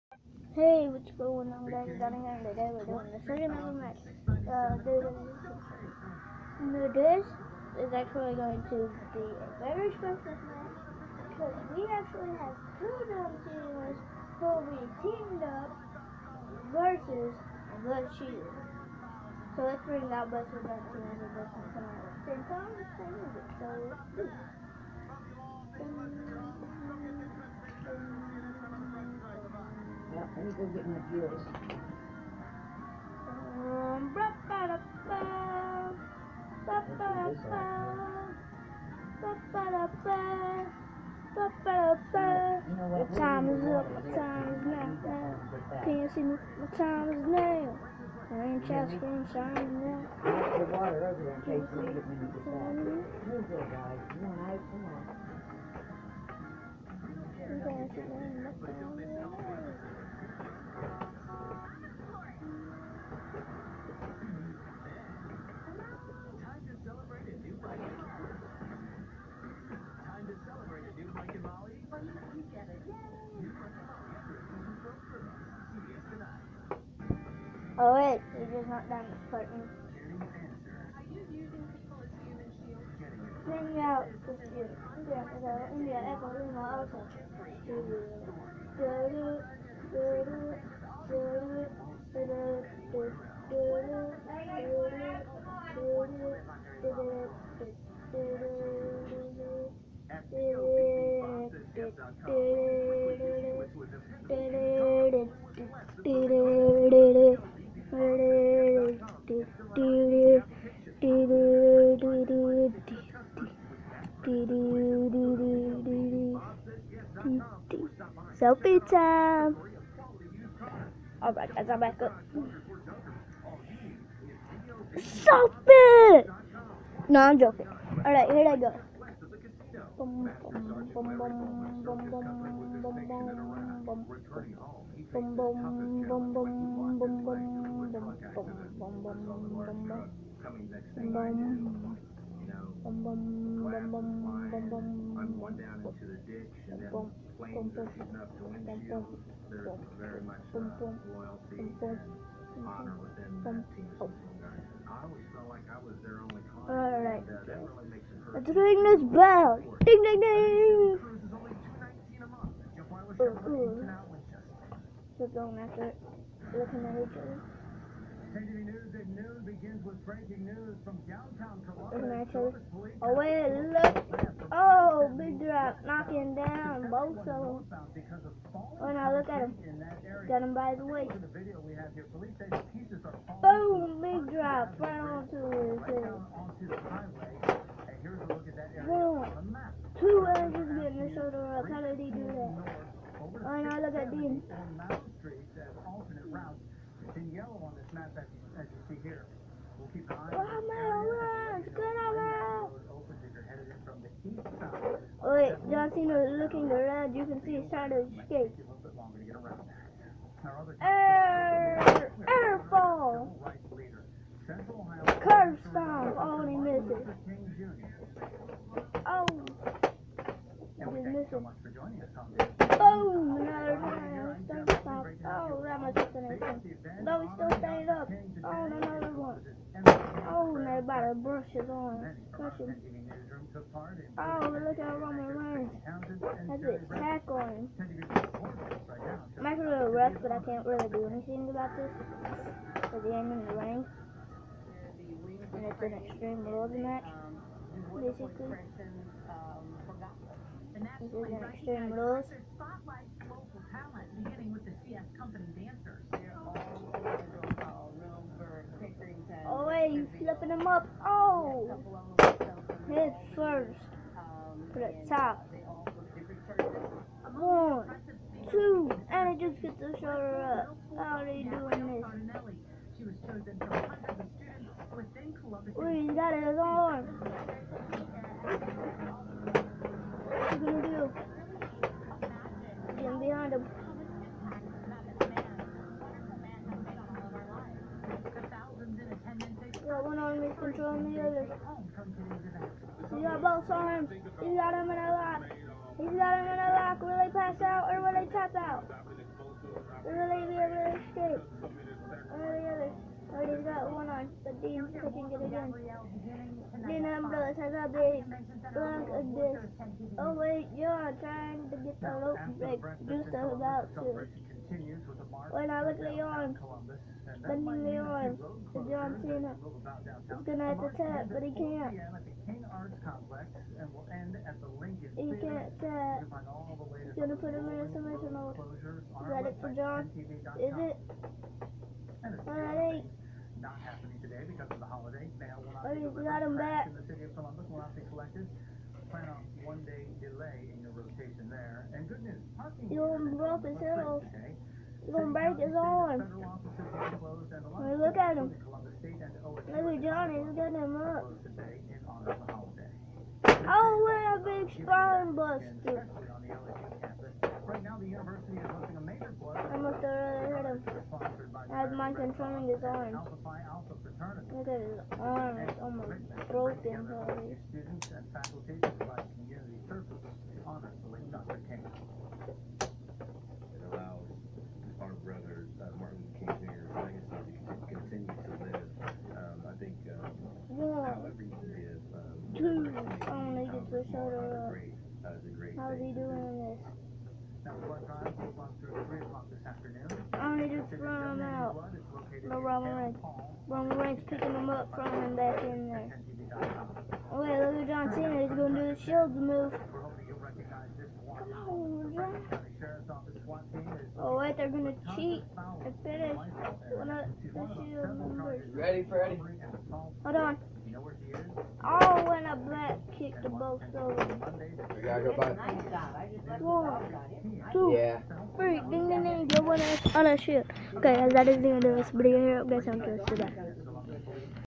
Second match with "WWE Super Strikers"
Includes 2 John Cena's and The Shield (only Roman Reigns and Dean Ambrose).Is a tag team match (not for the title because we haven't made them yet).